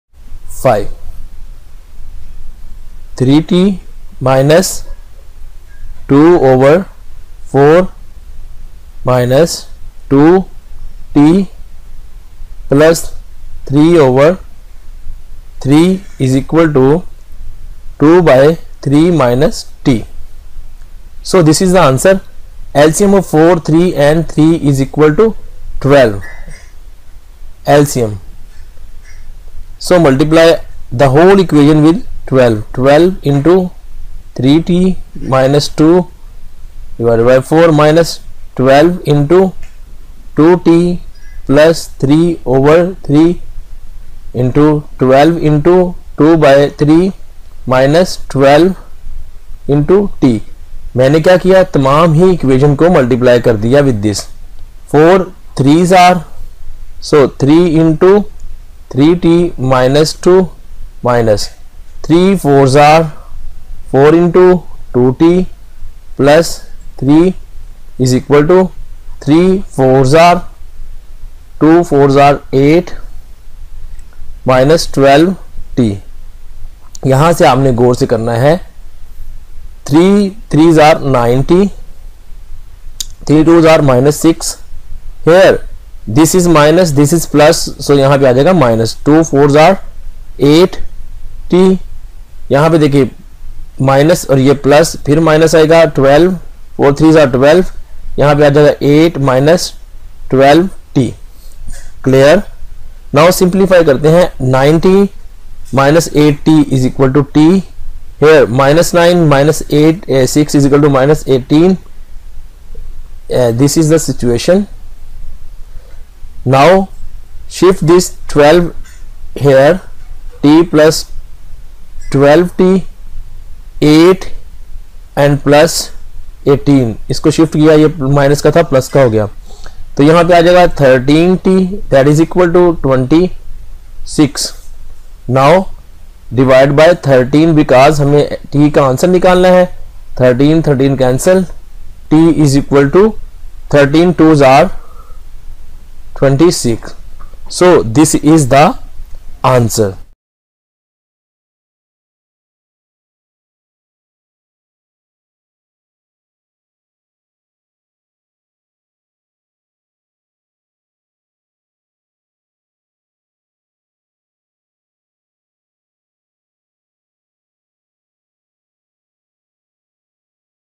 Q 5, Ex 2.5 Linear Equations in One Variable || NCERT Math Class 8 || Solve and Check the Equations
How to Solve Linear Equations in One Variable and Check || Maths Grade 8 || Mathematics for Class 8 CBSC

Skype : muhammadhshiad01

Face Book Page : Technical Mathvbn